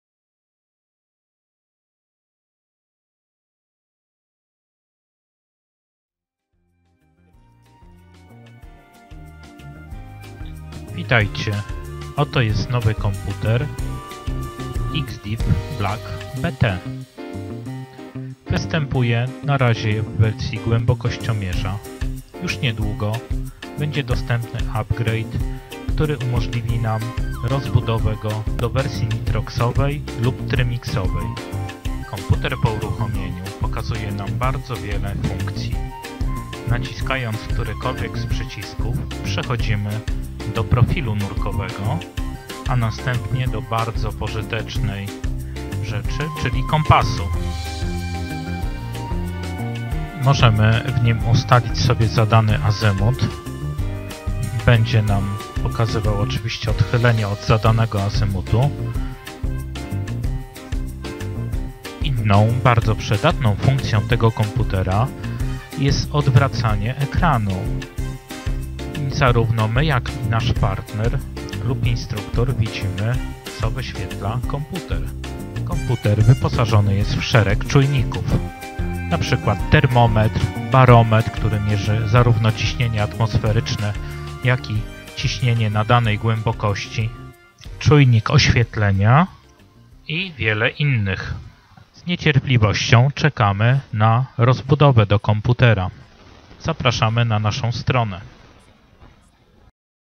XDeep BLACK BT prezentacja
nurtech.pl

Komputer nurkowy XDeep BLACK BT - krótka prezentacja możliwości, zaraz po rozpakowaniu, z pierwszej dostawy :)